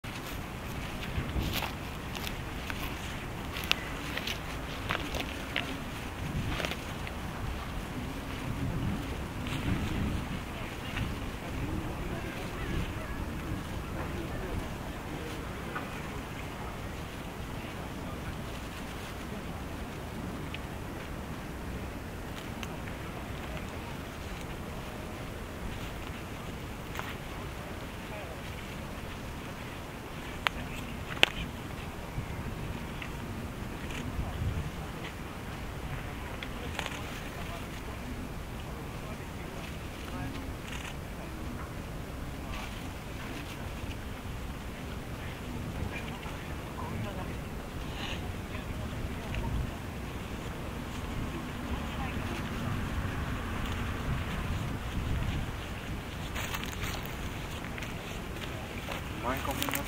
野鳥撮影・ ユキホオジロ♂ 3/3 Snow Bunting
千葉県でユキホオジロと出会いました。